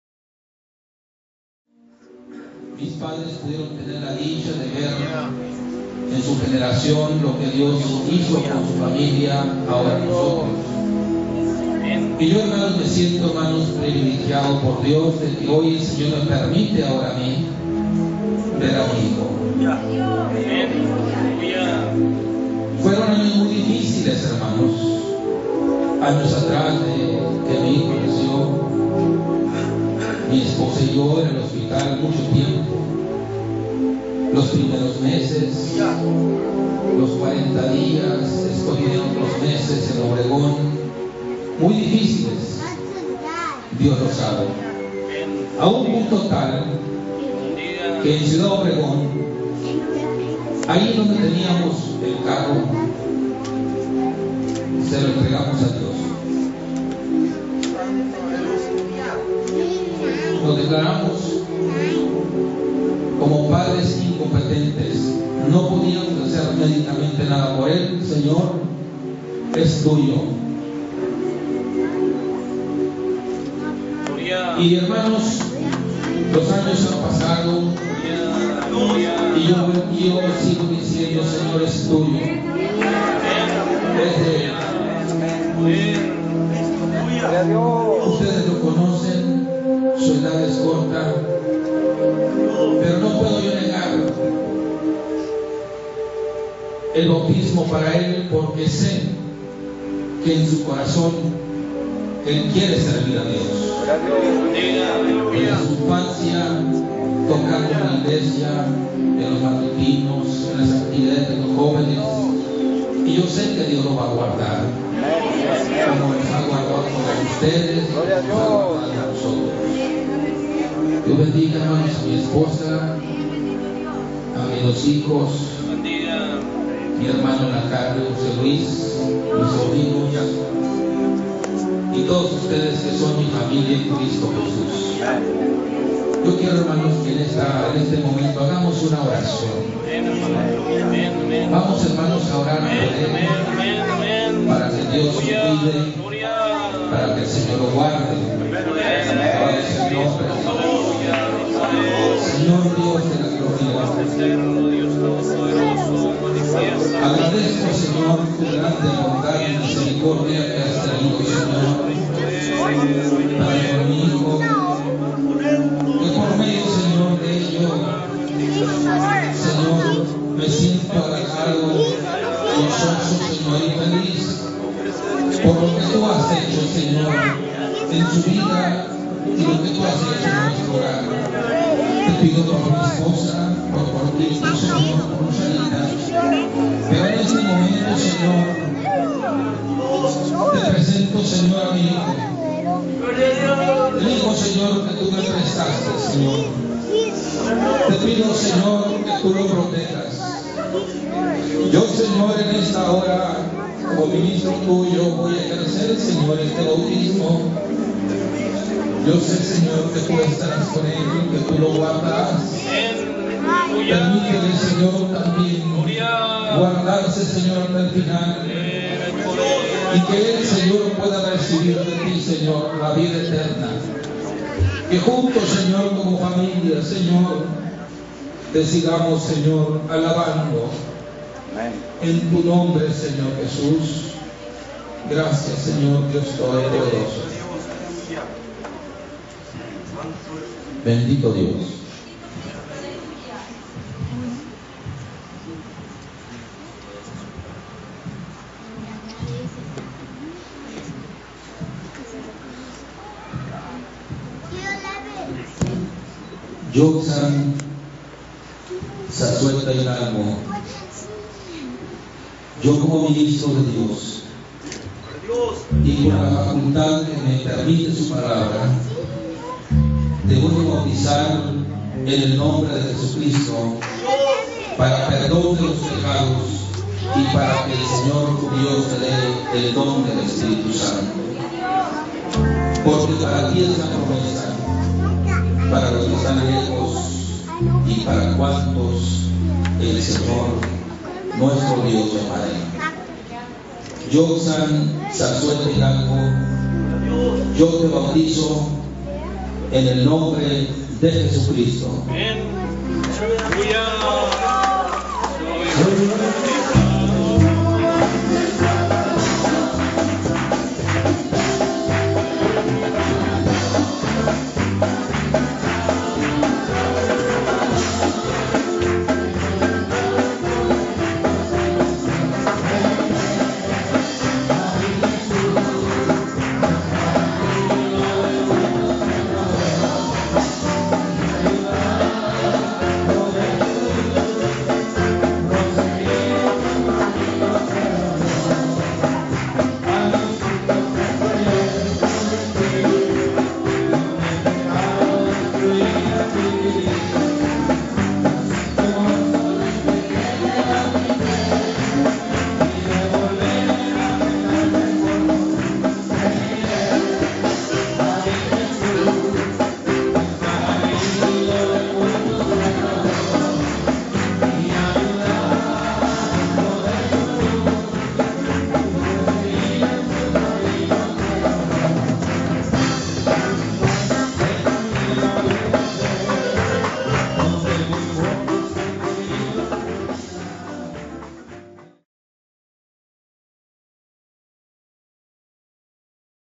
Bautismo de Joksan Zazueta
Nuestro hermano Jorge Zazueta, tiene el prebilejio de bautisar a su hijo Joksan.